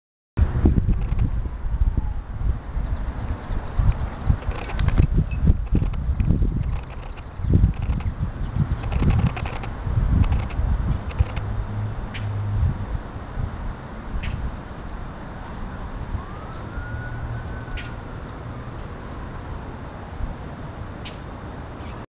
7 07 29 2012 LOTS OF HUMMING BIRDS
There are so many Hummingbirds now. It has been about 100 degrees here for 8 days. They come and feed at different times the day. The best times are before Sunset when they all come and feed before it gets dark. I have between 10 and 20 that come around then.